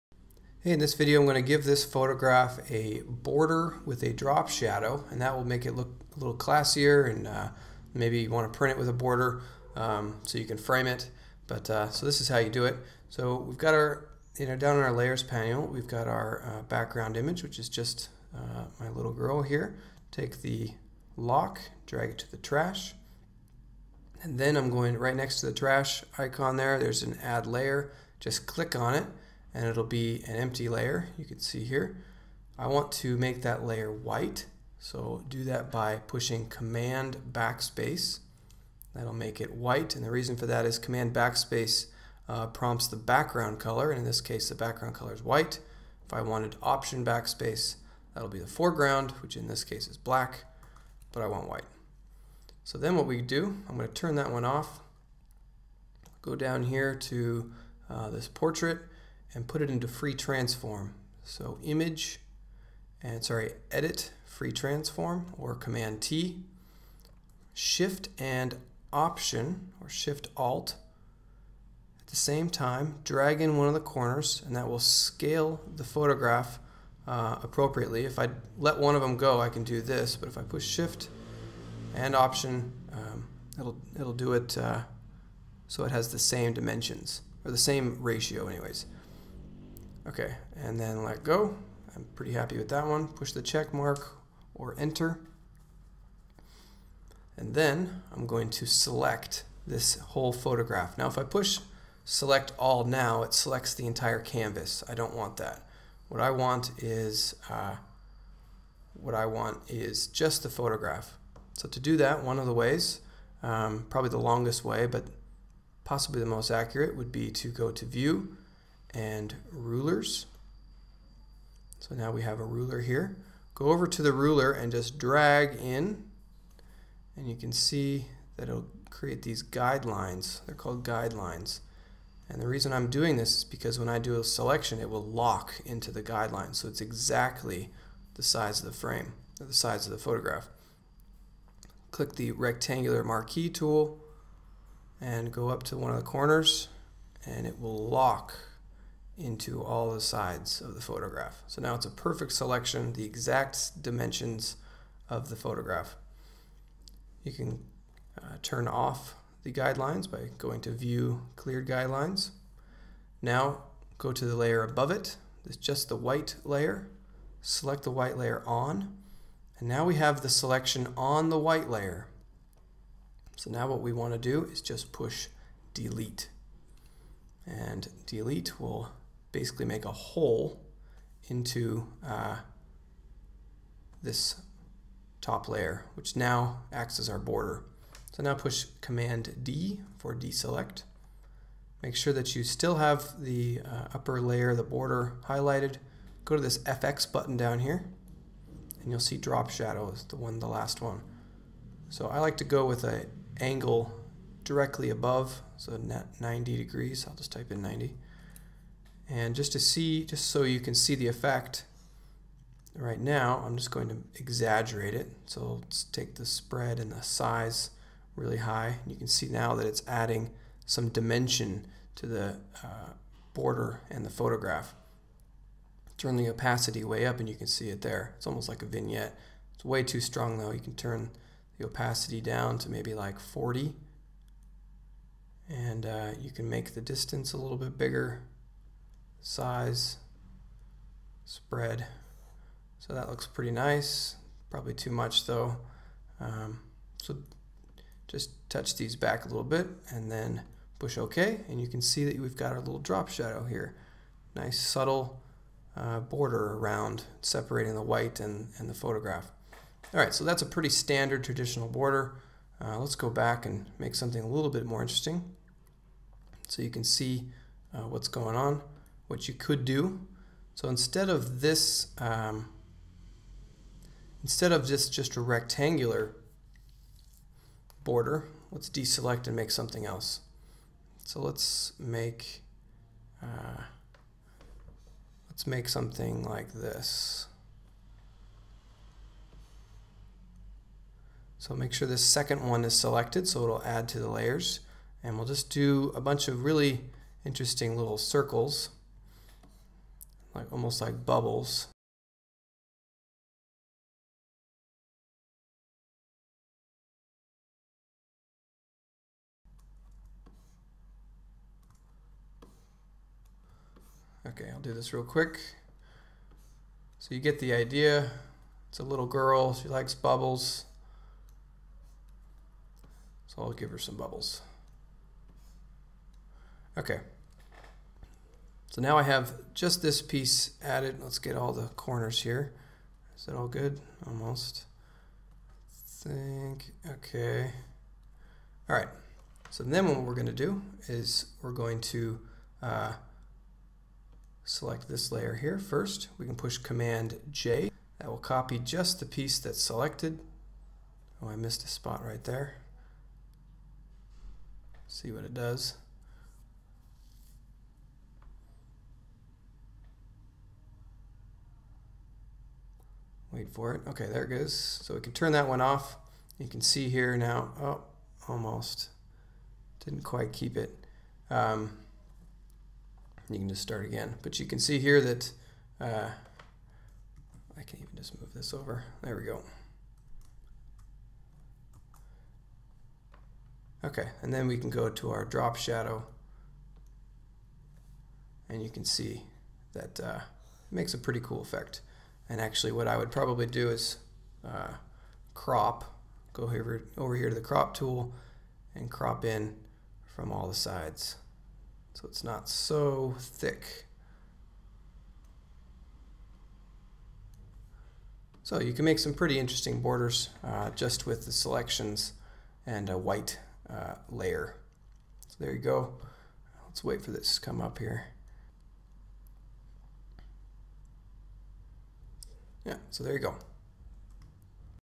adding a border to your photographs CS6
How to add a border and drop drop shadow to your photos in CS6